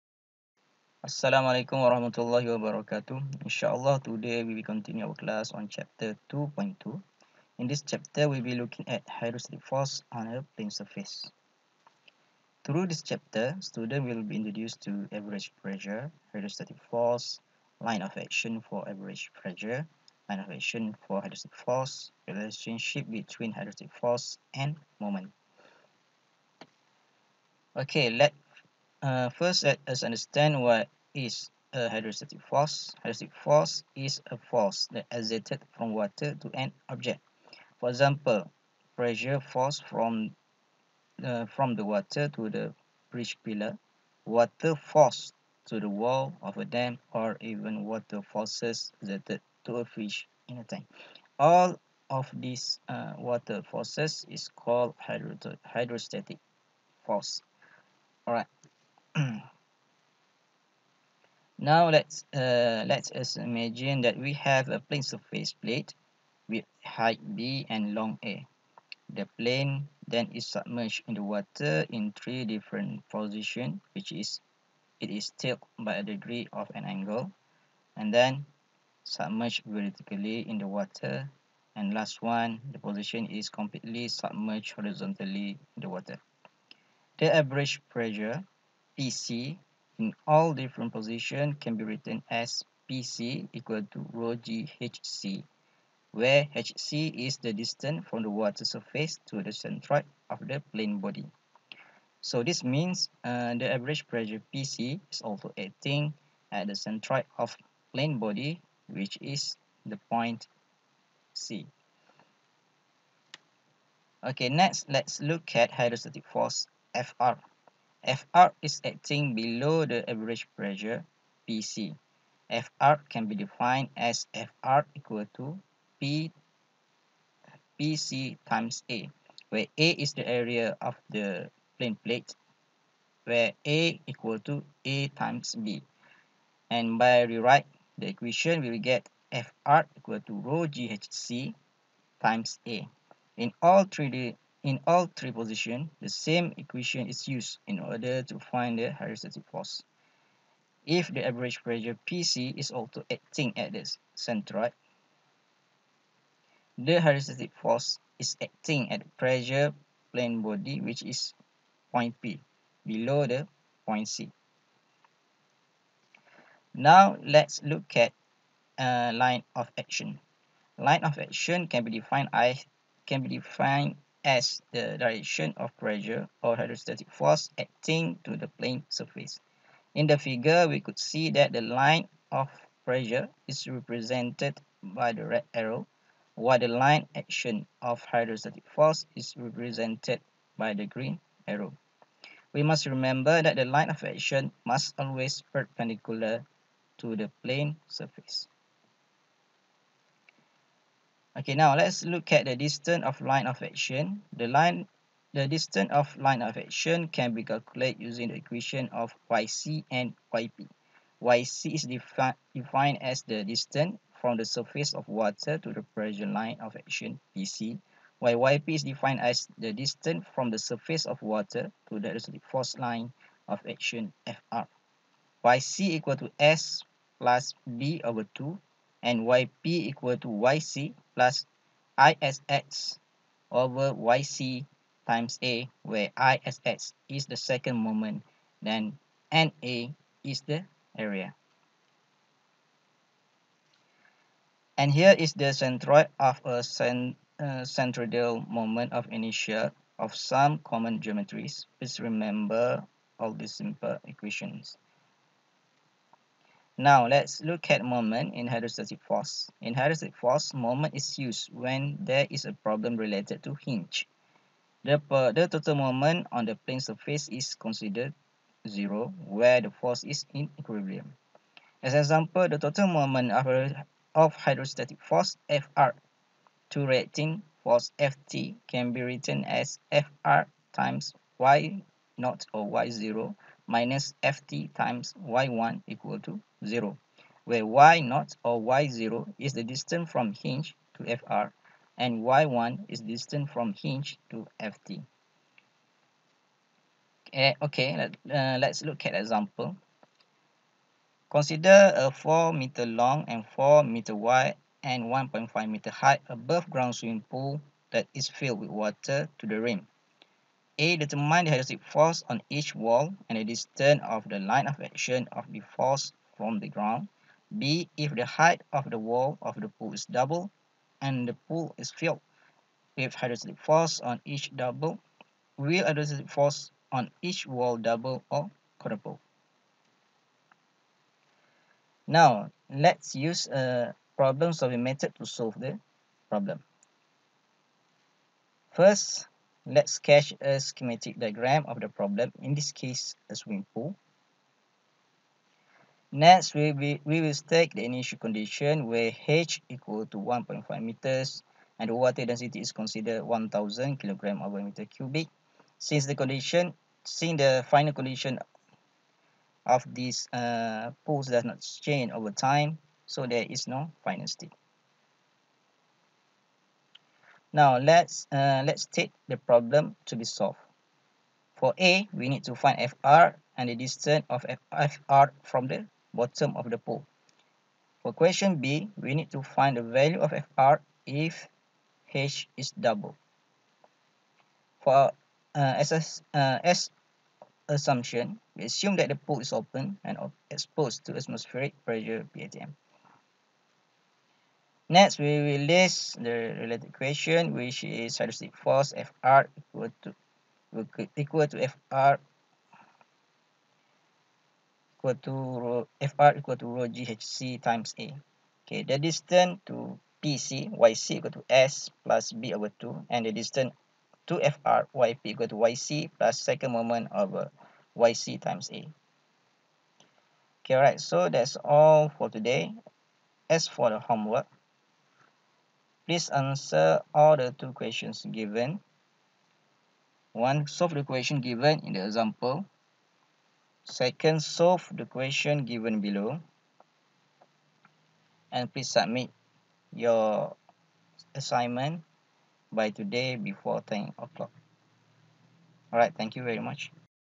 MEC241: Hydrostatic forces on plane surfaces (5/28)
In this chapter, the student will be introduced to:
1. Average pressure,
2. Hydrostatic force
3. Line of action for average pressure
4. Line of action for hydrostatic force
5. Relationship between hydrostatic force and moment